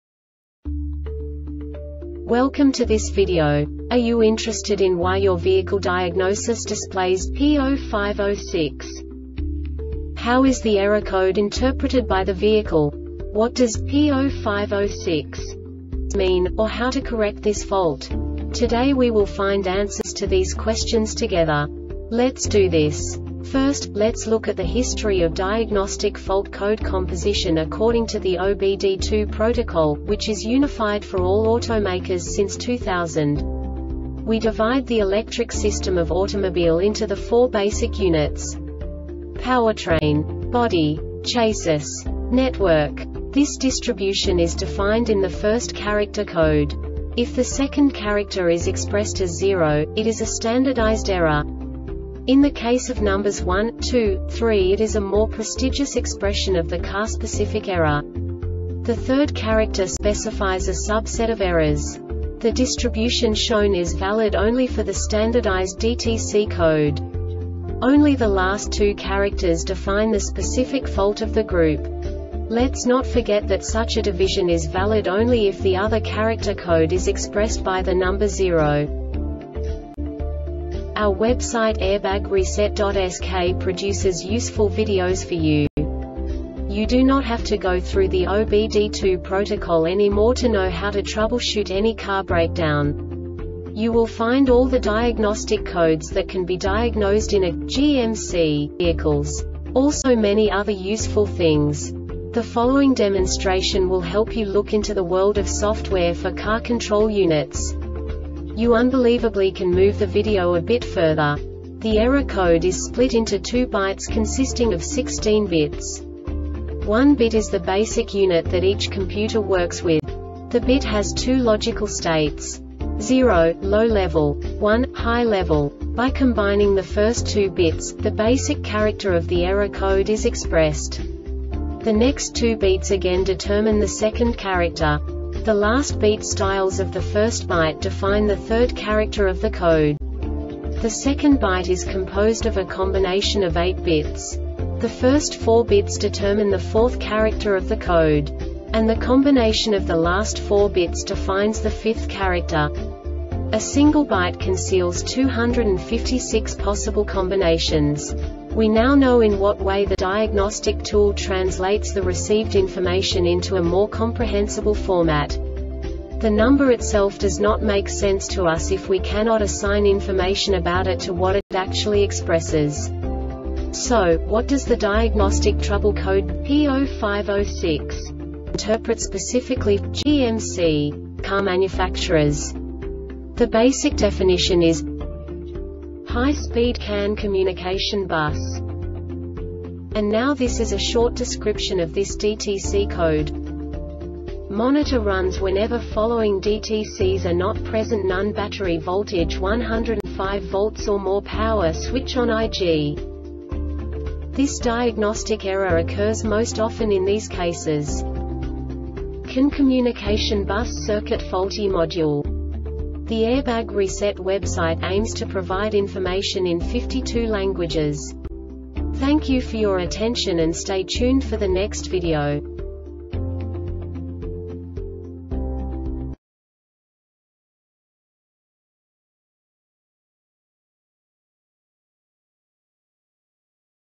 DTC GMC P0506 Short Explanation
Contents:
0:21 Basic DTC analysis according to OBD2 protocol standard.
1:48 Insight into programming
2:58 A diagnostically understandable description of the Diagnostic Trouble Code
3:05 Basic error definition

Make:
GMC
Code:
P0506
Definition:
High Speed CAN Communication Bus
Description: